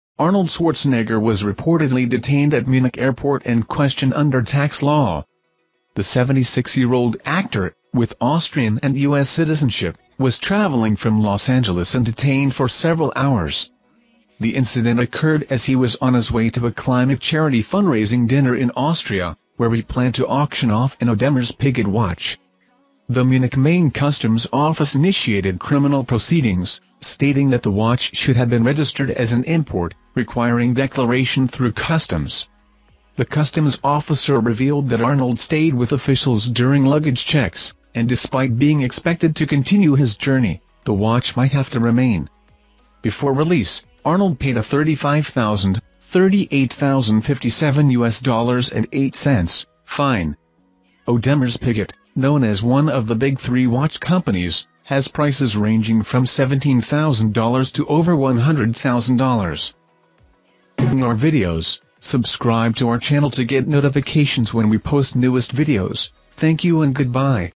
Arnold Schwarzenegger ‘detained’ at Munich Airport over ‘unregistered luxury watch’
Arnold Schwarzenegger has reportedly been ”detained” at Munich Airport and was questioned under “tax law,” according to German media.